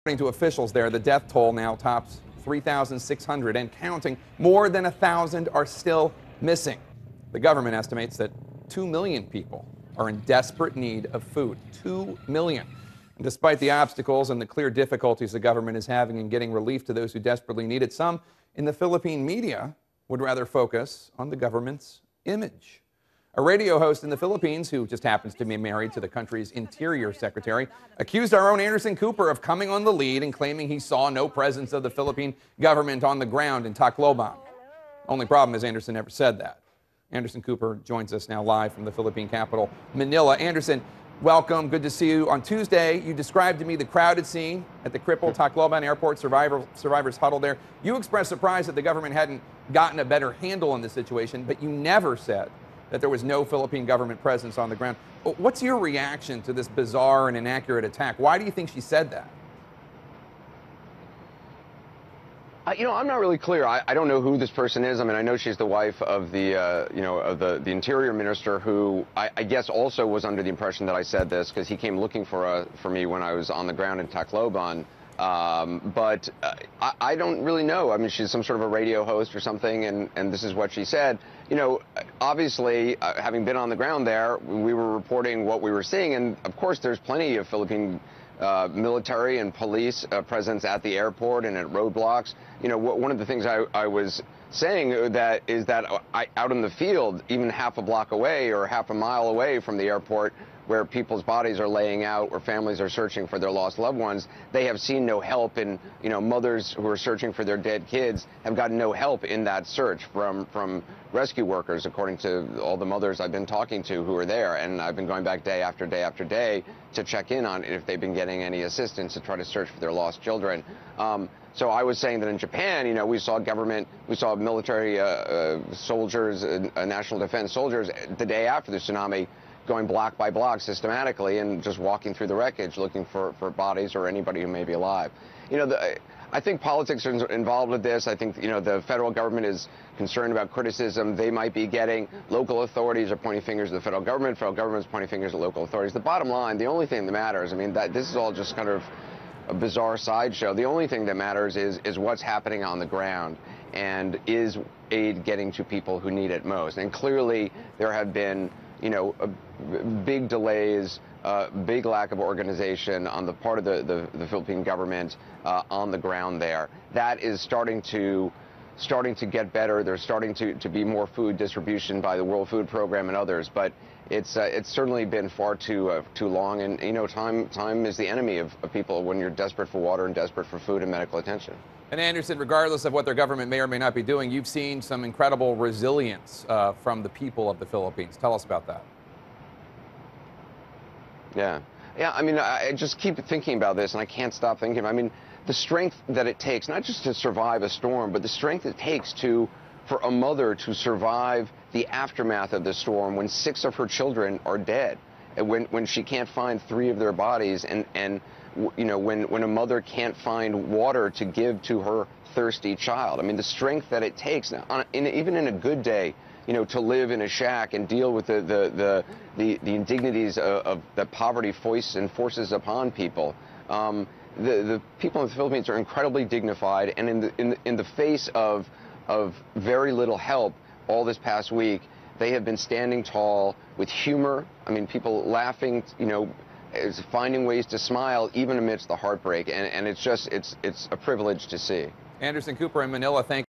CNN's Anderson Cooper defends coverage
Filipina host attacked CNN's Anderson Cooper for something he never said while covering Typhoon Haiyan's aftermath.